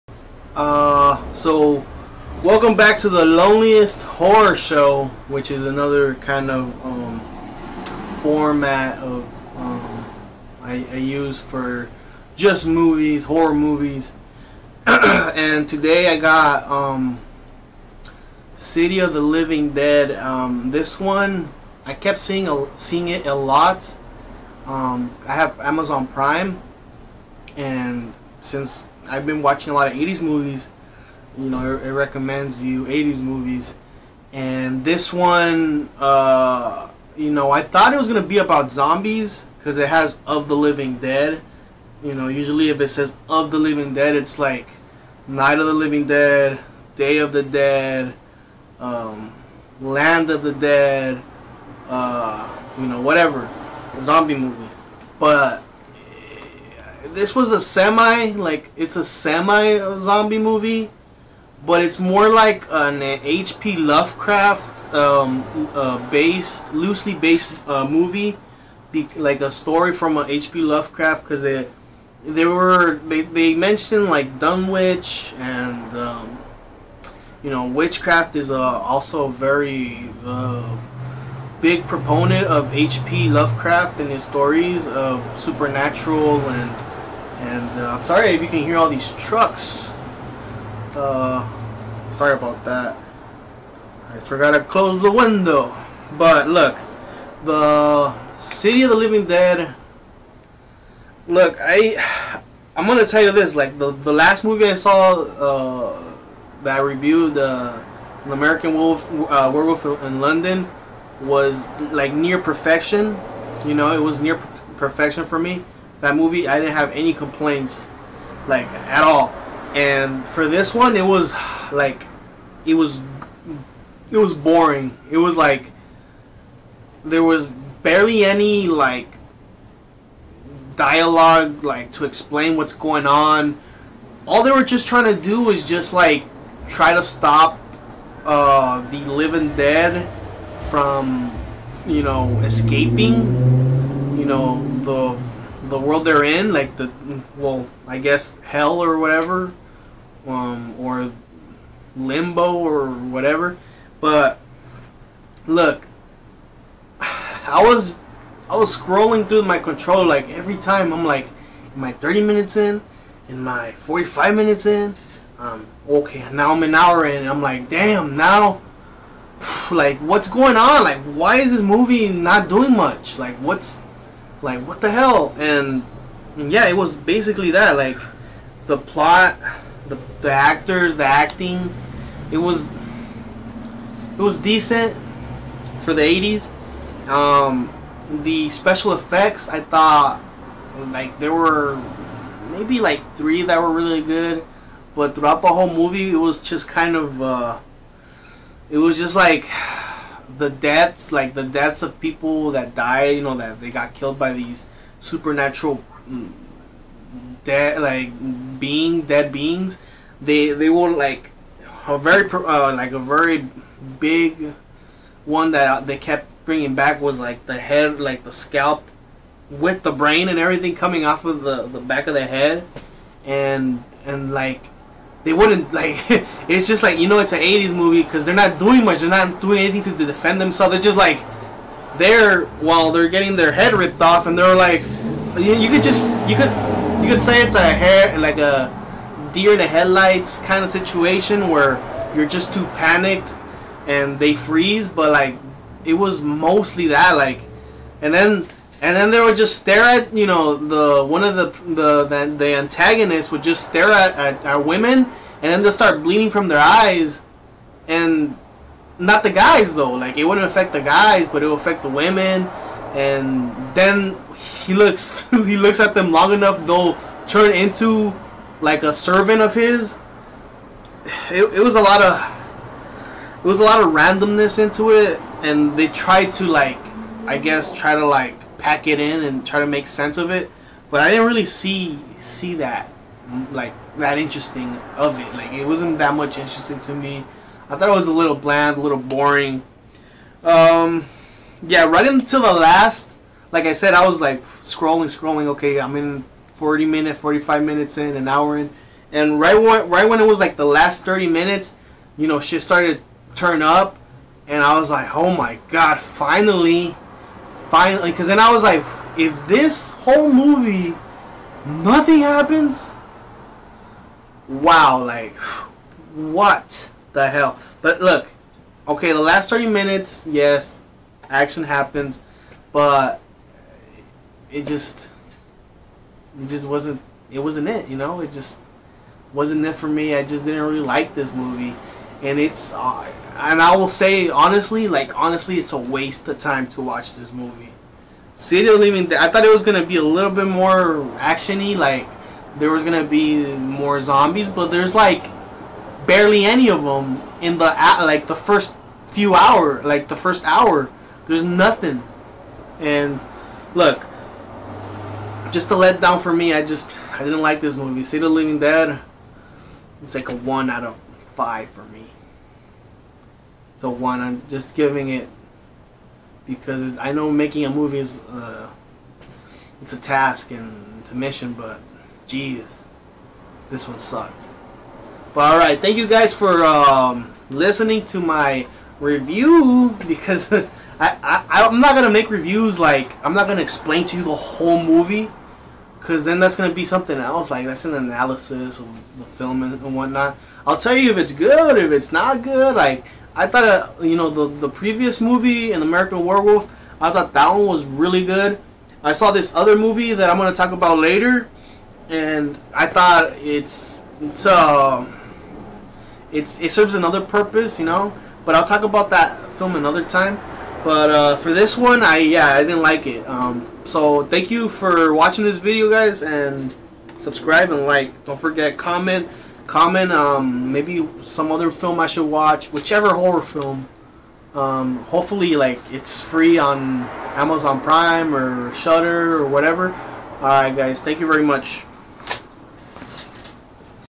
City Of The Living Dead Review | The Loneliest Horror Show 2
I apologize for the audio issues. City Of The Living Dead(1980) Didn't like this film very much. But here it its.